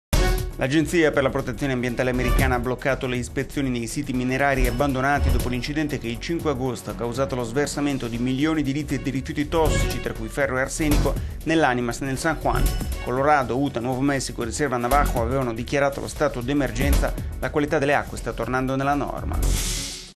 Colorado. Acque tornano nella norma dopo sversamento rifiuti minerari
L'Agenzia per la Protezione Ambientale americana (Epa) ha bloccato le ispezioni nei siti minerari abbandonati dopo l'incidente che il 5 agosto ha causato lo sversamento di milioni di litri di rifiuti tossici tra cui ferro e arsenico nell'Animas e nel San Juan. Colorado, Utah, Nuovo Messico, Riserva Navajo avevano dichiarato lo stato d'emergenza. La qualità delle acque è ora rientata nella norma.…